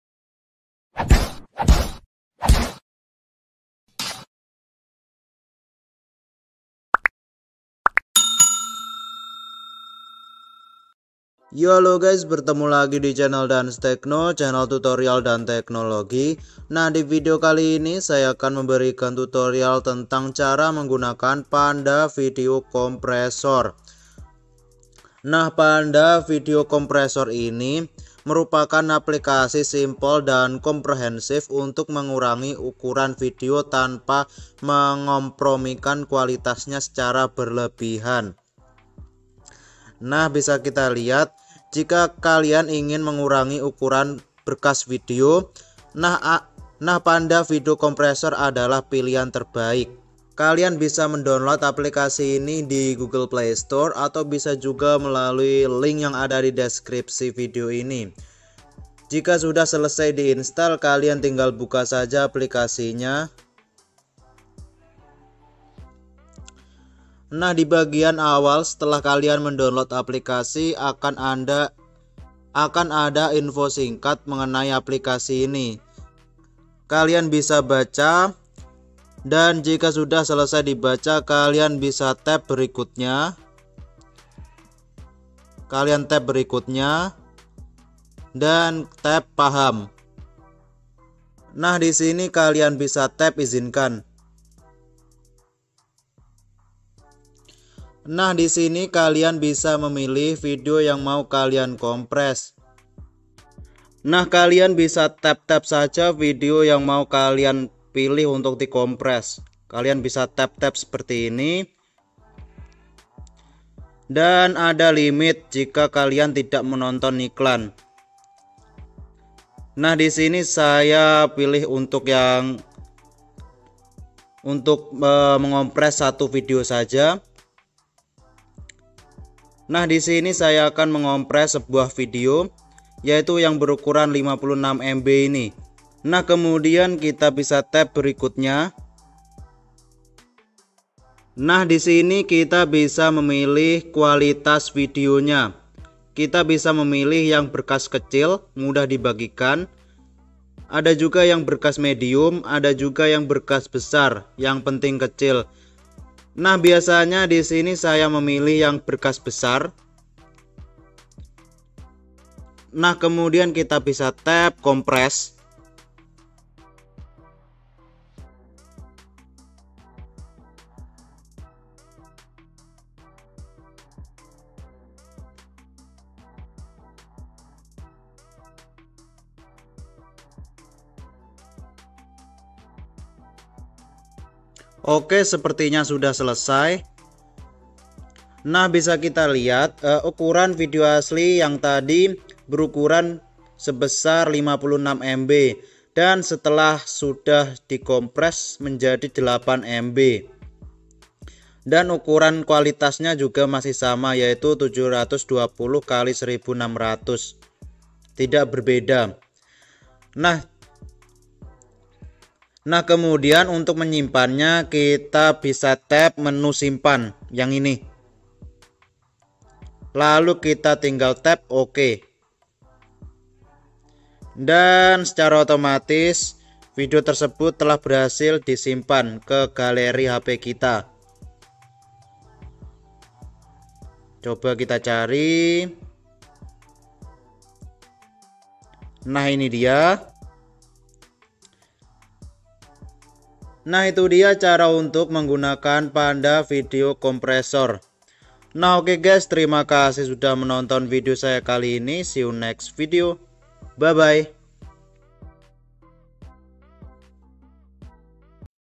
Cara Menggunakan Panda Video Compressor
✨Hallo guys✨
Di video kali ini saya akan memberikan tutorial tentang Cara Menggunakan Panda Video Compressor
😎 . Buat kalian yg penasaran gimana caranya, tonton video ini sampai habis. Ok👌

Happy Watching🤗

•Record: xrecorder
•Thumbnail: Pixellab
•Editing: openshot
•Background video: